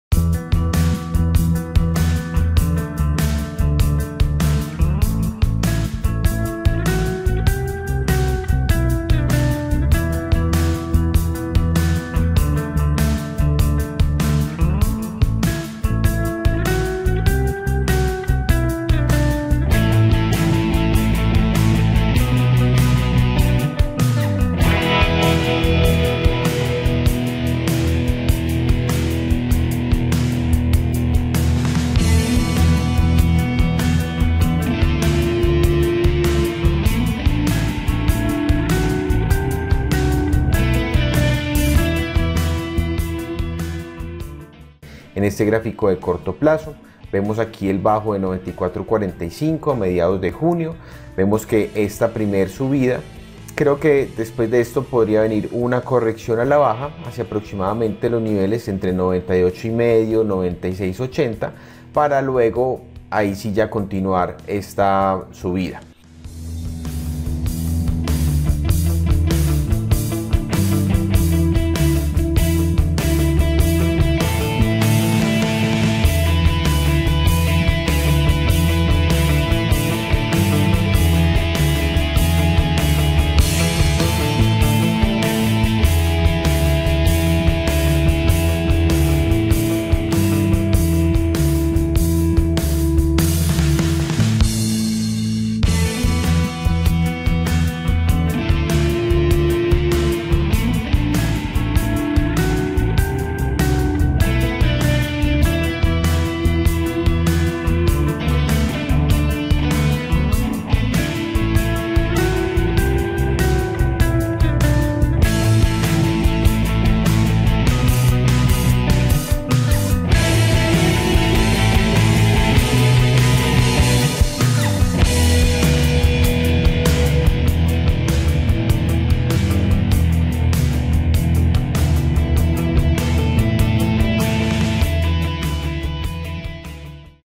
Cómo Aprovechar la Tendencia Actual del Precio del Café
La alta volatilidad en los precios del café está para quedarse... Y mientras algunos se estresan por no saber lo que sucederá, otros aprovechan las tendencias a su favor e incrementan las utilidades de su negocio cafetero y de sus inversiones!

Conoce cómo puedes recibir nuestros análisis de primera mano y anticiparte a lo que vendrá en el precio del café